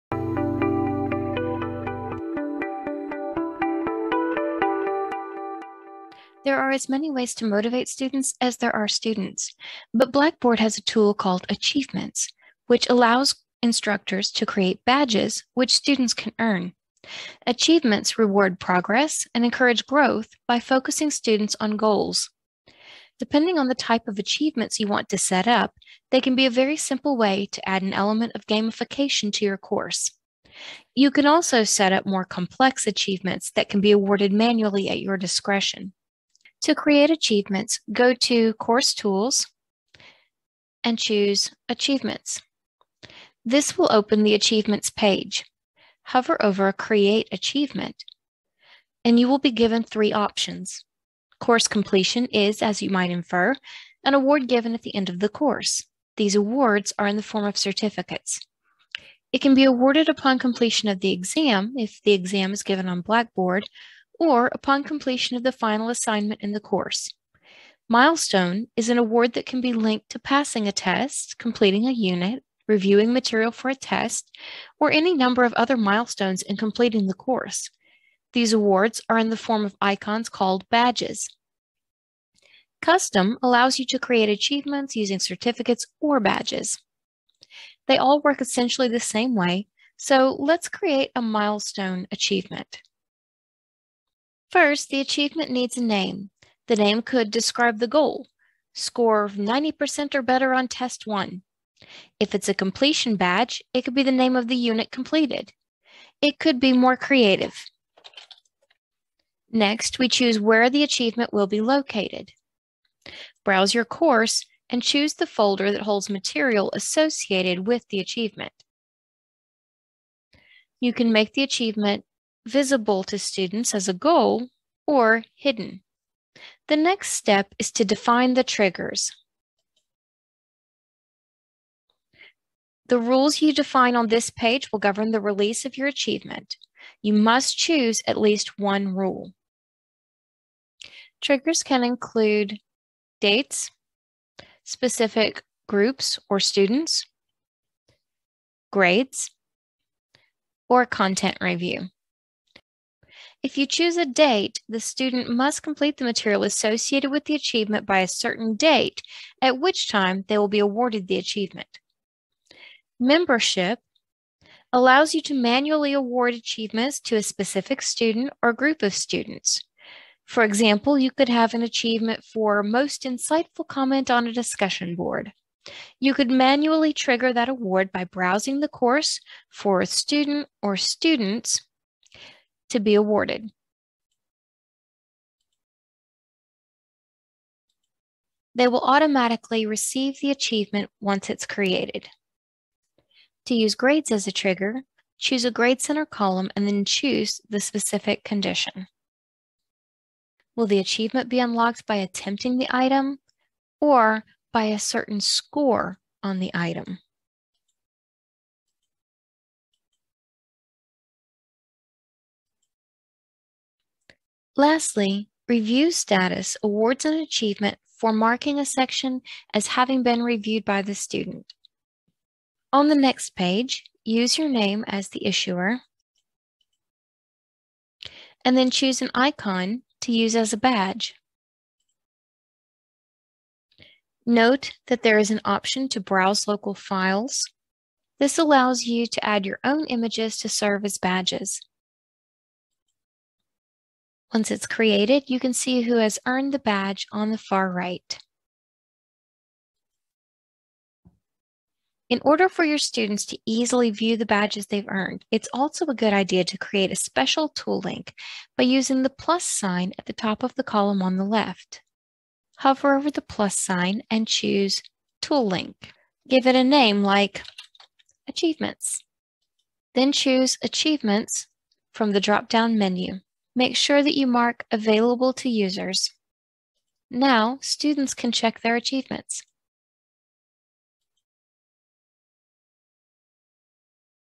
Achievements in Blackboard
This video explains how to create achievement certificates and badges in Blackboard.